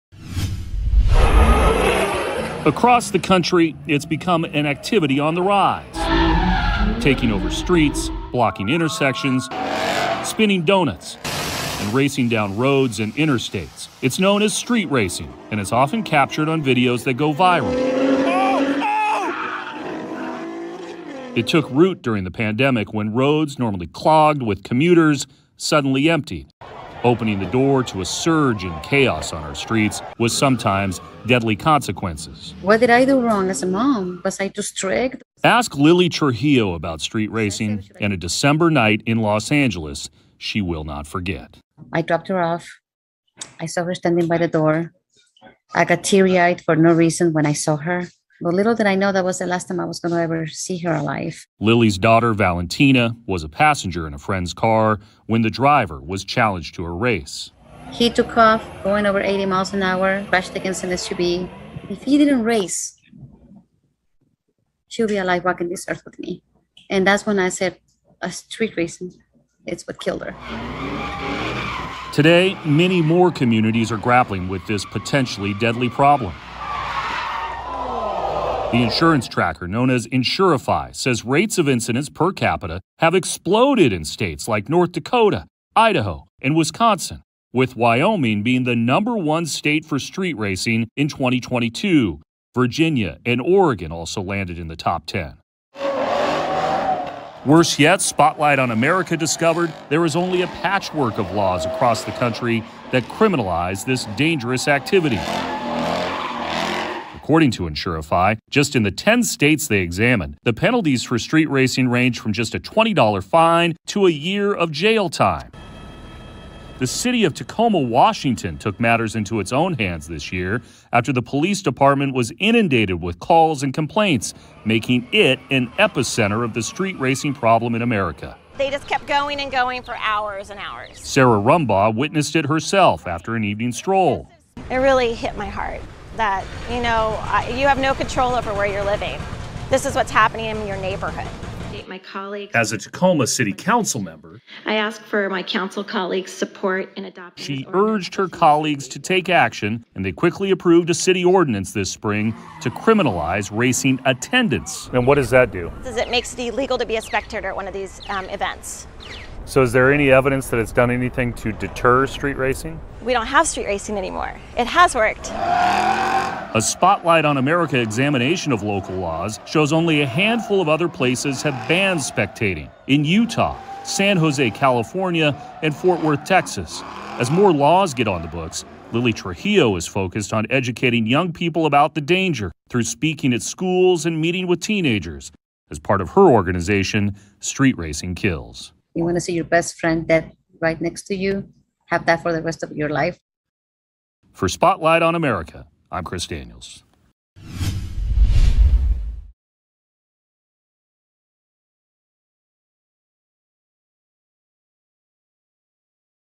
Street racers are taking over roads with deadly consequences as laws struggle to keep up
Illegal car races are invading American streets from coast to coast, with shocking video emerging of huge crowds gathered to watch highly dangerous driving.

The consequences can be deadly and can terrorize neighborhoods. Despite this growing problem, Spotlight on America has discovered there's only a patchwork of laws and strategies to reign in street racing across the US. Communities have deployed strategies from adding speed bumps, closing roads, even putting out decoy police cars.

Experts say more needs to be done to ensure safety on our streets.

Street racing is an activity on the rise, from Baltimore and Portland to Seattle and Salt Lake City, and many more communities all across America. Chicago recently formed a task force to try to tackle the problem. Just this past month, Phoenix police said four people were killed as a result of street racing.

The issue took root during the pandemic, when roads normally clogged with commuters suddenly emptied, opening the door to a surge in illegal street racing.